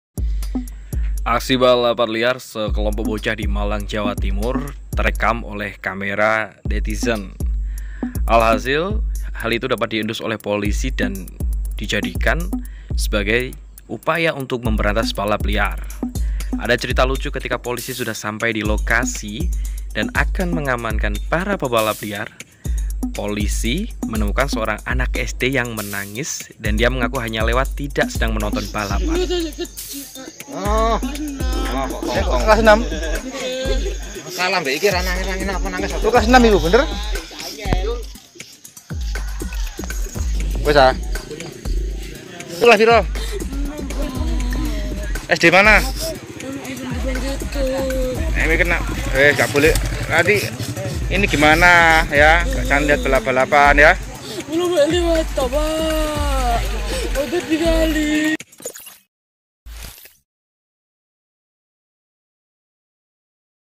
SOSOK BOCAH NANGIS TERISAK SAAT POLISI DATANGI ARENA BALAP LIAR DI MALANG : SAYA CUMA LEWAT, PAK
Kejadian lucu terjadi saat polisi hendak merazia para pebalap liar di Malang. Seorang bocah menangis ketakutan saat melihat polisi datang.

[editor:aj]